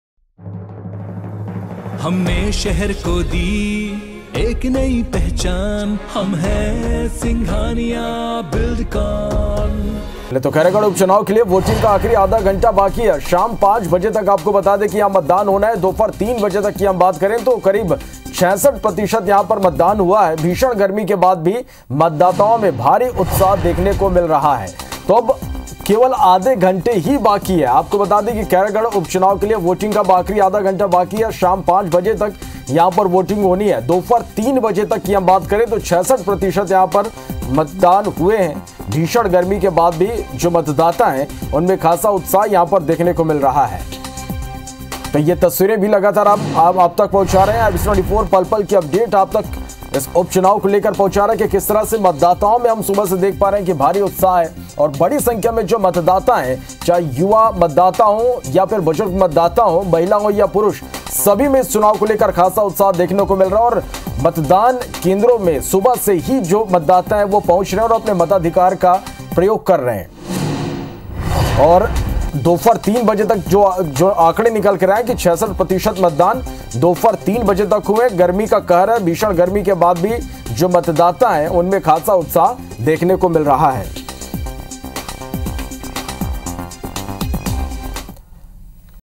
Khairagarh By-Election : दोपहर 3 बजे तक करीब 66% मतदान | भीषण गर्मी के बाद भी वोटर्स में भारी उत्साह
IBC24, Khairagarh Assembly By-Election 2022 , दोपहर 3 बजे तक करीब 66% मतदान, भीषण गर्मी के बाद भी वोटर्स में भारी उत्साह